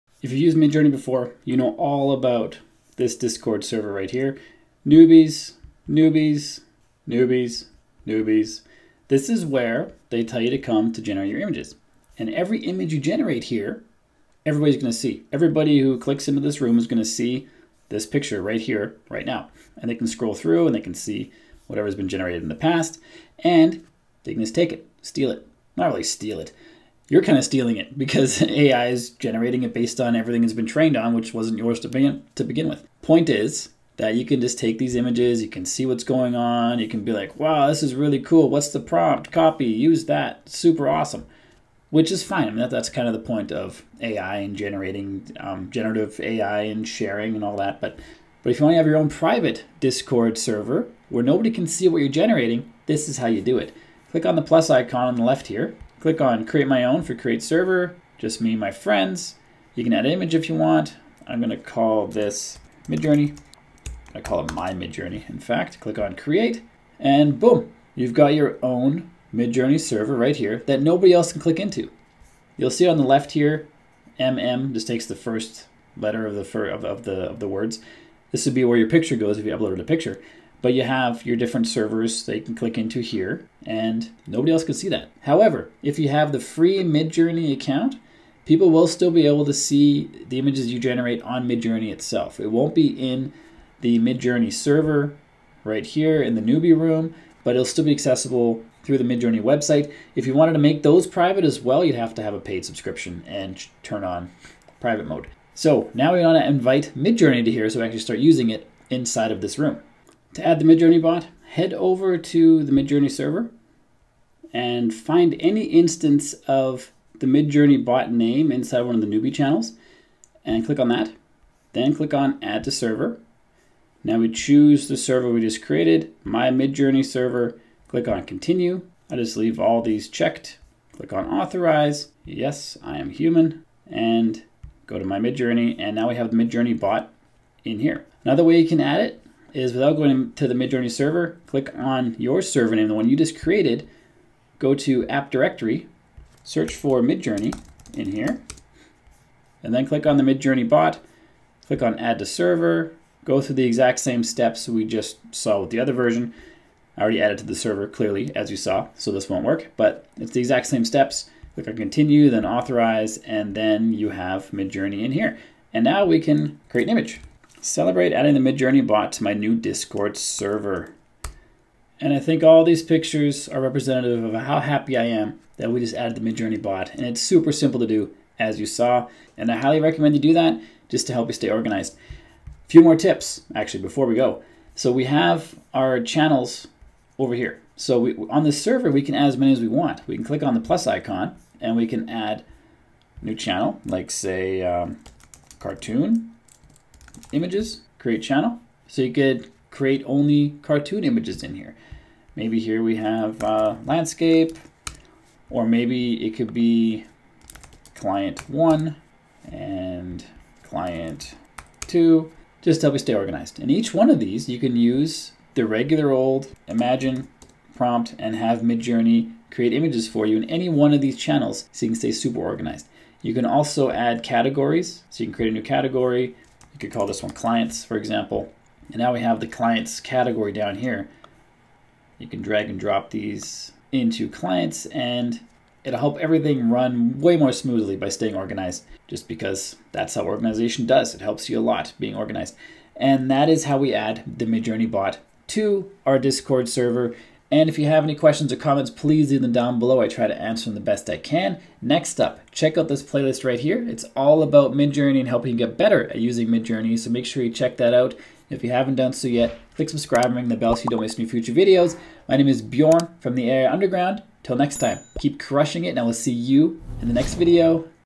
How to create a PRIVATE Midjourney server in Discord - The quick and easy way
When you create an image on Midjourney, it's available for everyone in the same channel to see. But, did you know you can create your own private Midjourney servers in Discord? You can and it hides the images from the main channels in Discord!

How to Setup Your Own Discord Server with the Mid Journey Bot 🤖

Hello, fellow tech enthusiasts! It's Bjorn from the Area Underground, and today I'm thrilled to guide you through setting up your own private Discord server with the fantastic Mid Journey bot. Whether you're just starting your journey or you're a seasoned user, you're in the right place.
Why Mid Journey on Discord?

Every time you generate an image on the common Mid Journey Discord server, it's displayed for all members to see. While this offers a space for sharing and collaboration, it might not suit those looking for a bit more privacy. 😌
Setting Up Your Private Server

Starting is as simple as:

    Clicking on the plus icon on your Discord.
    Selecting "Create My Own" for creating a server.
    Naming it (I've called mine 'Mid Journey') and adding an image if desired.

Voila! 🎉 You have your own server which others cannot access unless invited. But remember, if you're using a free Mid Journey account, the images you generate might still be visible on the Mid Journey website. For absolute privacy, consider upgrading to a paid subscription.
Inviting the Mid Journey Bot

To get the most out of your new server, you'll want to invite the Mid Journey bot. Here’s how:

    Go to the Mid Journey server.
    Find the Mid Journey bot name inside a newbie channel.
    Click on 'Add to Server' and select your newly created server.

Alternatively, you can search for the Mid Journey bot in the app directory of your server and follow the same steps. 🔄
Organizing Your Channels for a Streamlined Experience

Your new server isn’t limited to just one channel. By clicking on the plus icon, you can create multiple channels such as 'cartoon images' or specific channels for different clients. For better organization, consider categorizing channels. This helps in ensuring that everything runs smoothly and makes your experience more enjoyable. 📁

Wrap Up
Incorporating the Mid Journey bot into your Discord server allows you to harness the power of AI and generate a multitude of images. With organization tips under your belt, you're all set for a seamless experience.

If you've got any questions, feel free to drop a comment! 🛎️

Until next time, keep crushing it! 🚀